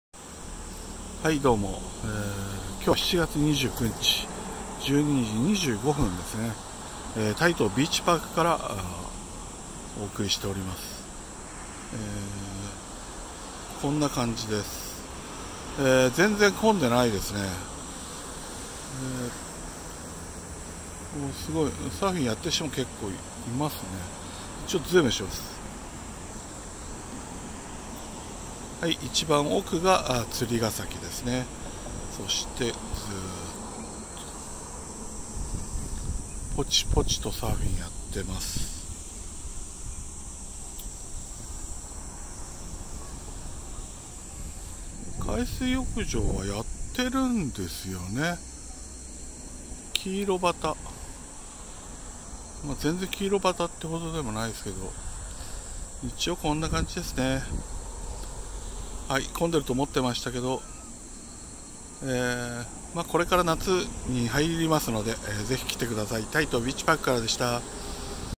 2019/7/29 太東ビーチパーク情報(東京オリンピックサーフィン会場より徒歩10分)12:25
釣ヶ崎はオリンピックまで工事が続くとの事なので、
徒歩10分の太東ビーチパークの波の模様をお伝えします。

頻繁に更新する予定ですので、あえて無編集で投稿します。
こんな風に撮ってほしいとかご希望があれば、出来る範囲で努力いたします。
コメント待ってます。

Pixel 3 xlで撮影しています